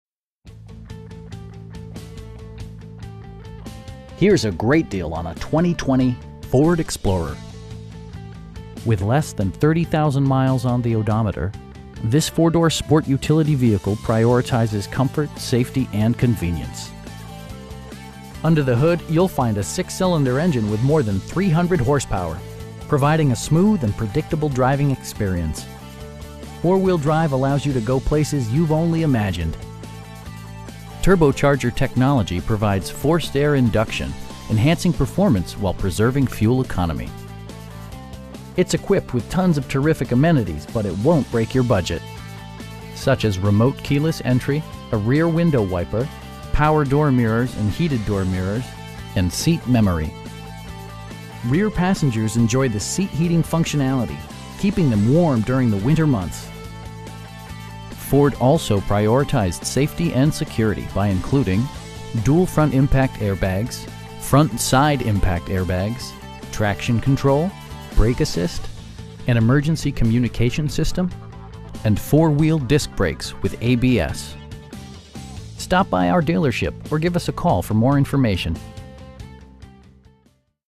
2020 Ford Explorer ST in Moline, IL 61265
Sexton Ford
3802 16th Street

Here's a great deal on a 2020 Ford Explorer. With less than 30,000 miles on the odometer, this 4 door sport utility vehicle prioritizes comfort, safety and convenience. Under the hood you'll find a 6 cylinder engine with more than 300 horsepower, providing a smooth and predictable driving experience. Four wheel drive allows you to go places you've only imagined. Turbocharger technology provides forced air induction, enhancing performance while preserving fuel economy. It's equipped with tons of terrific amenities, but it won't break your budget. Such as remote keyless entry, leather upholstery, front and rear reading lights, a rear window wiper, power front seats, telescoping steering wheel, power door mirrors and heated door mirrors, and seat memory. Rear passengers enjoy the seat heating functionality, keeping them warm during the winter months. Ford also prioritized safety and security by including: dual front impact airbags, front side impact airbags, traction control, brake assist, a security system, an emergency communication system, and 4 wheel disc brakes with ABS. With electronic stability control supplementing mechanical systems, you'll maintain precise command of the roadway. We know that you have high expectations, and we enjoy the challenge of meeting and exceeding them! Stop by our dealership or give us a call for more information.